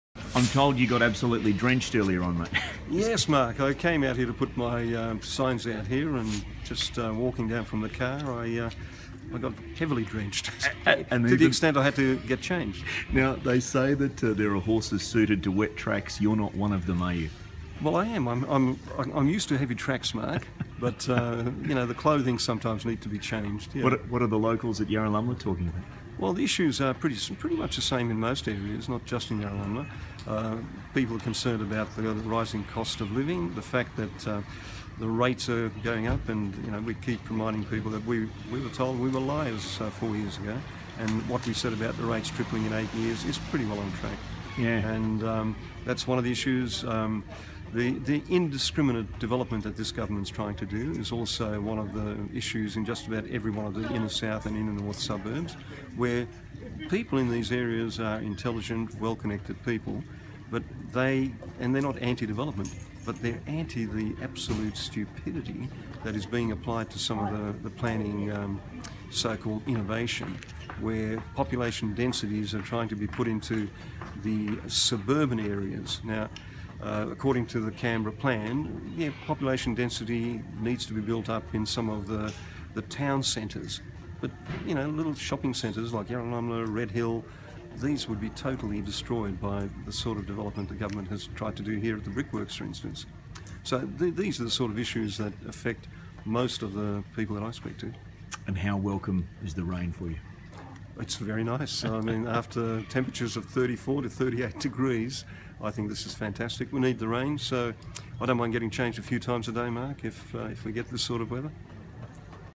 Steve Doszpot - Yarralumla shops
Talking about local issues at the Yarralumla shops.